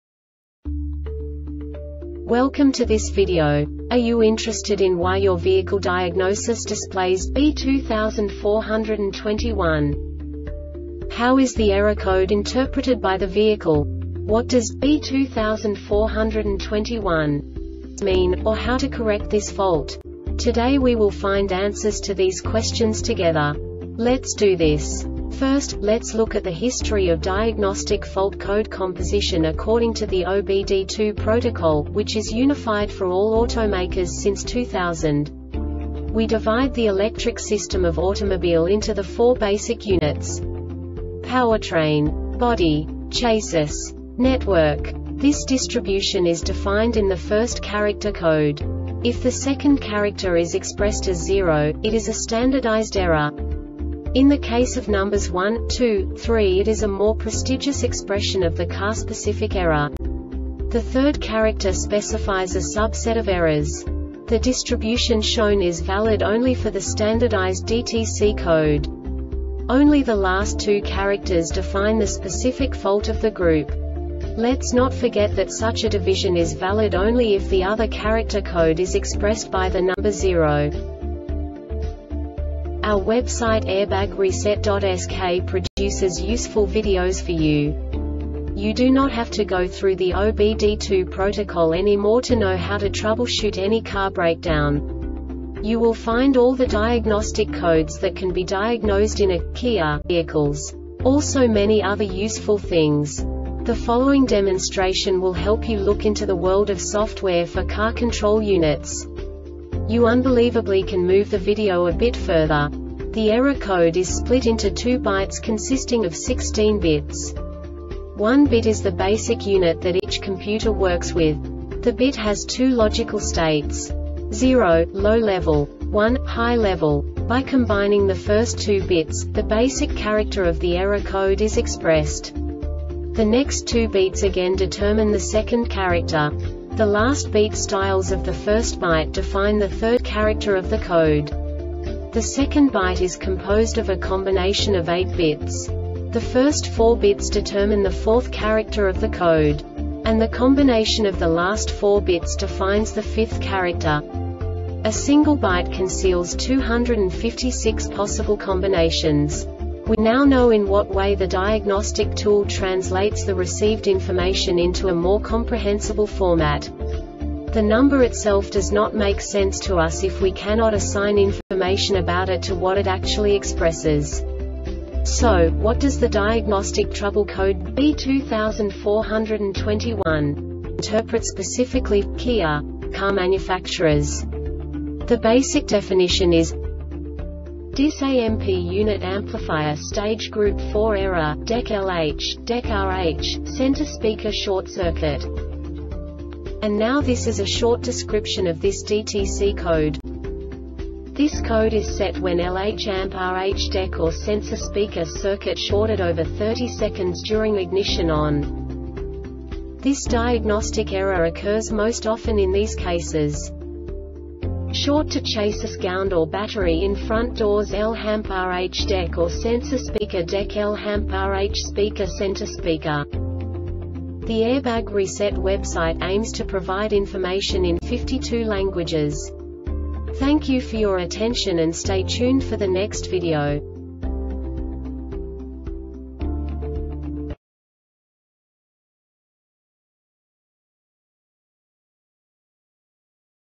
DTC KIA B2421 Short Explanation
Contents:
0:21 Basic DTC analysis according to OBD2 protocol standard.
1:48 Insight into programming
2:58 A diagnostically understandable description of the Diagnostic Trouble Code
3:05 Basic error definition

Make:
Kia
Code:
B2421
Definition:
DIS AMP Unit Amplifier stage group 4 Error - DECK LH, DECK RH, Center Speaker short circuit
Description:
This code is set when LH &amp;RH deck or sensor speaker circuit shorted over 30 sec during ignition on.
Cause:
 Short to chasis gound or battery in front doors (LH &amp;RH deck or sensor speaker)Deck LH &amp;RH speakerCenter speaker